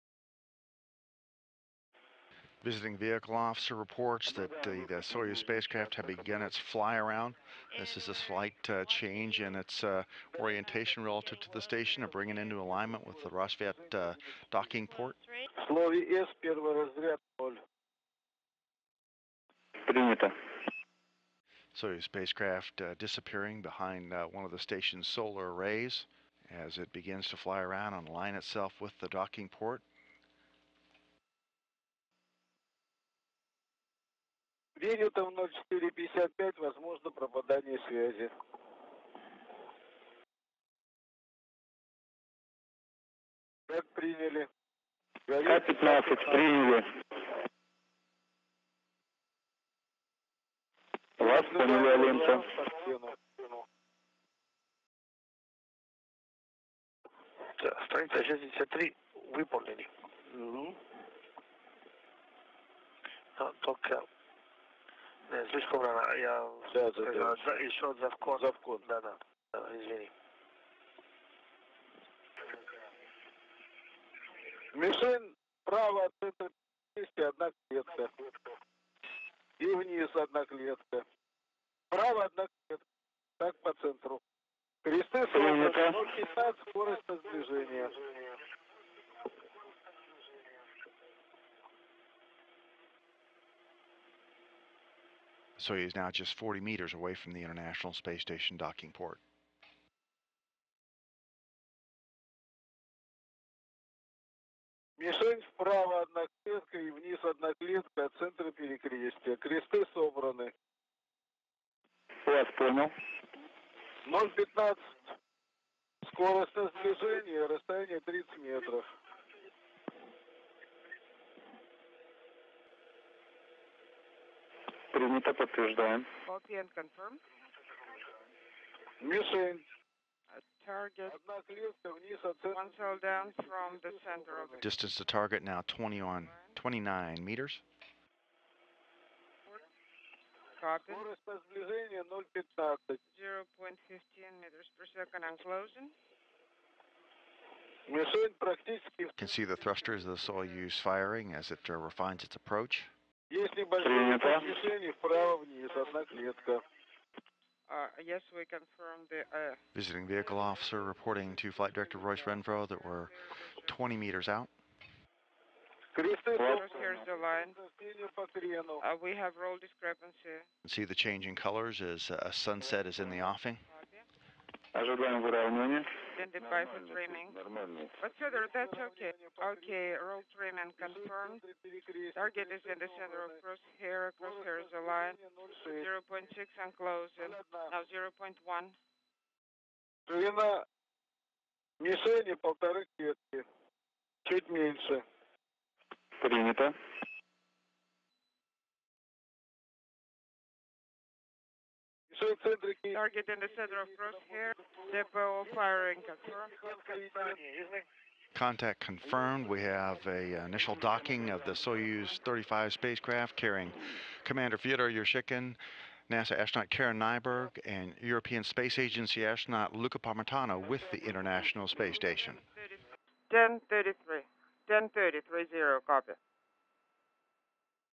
New Crew Reaches Space Station Six Hours After Launch | NASA Science Video
- Commander Fyodor Yurchikhin and Flight Engineers Karen Nyberg and Luca Parmitano launched from the Baikonur Cosmodrome in Kazakhstan aboard the Soyuz TMA-09M spacecraft, on a fast-paced 6-hour journey to the International Space Station, on May 28, 2013. Please rate and comment, thanks!
Credit: NASA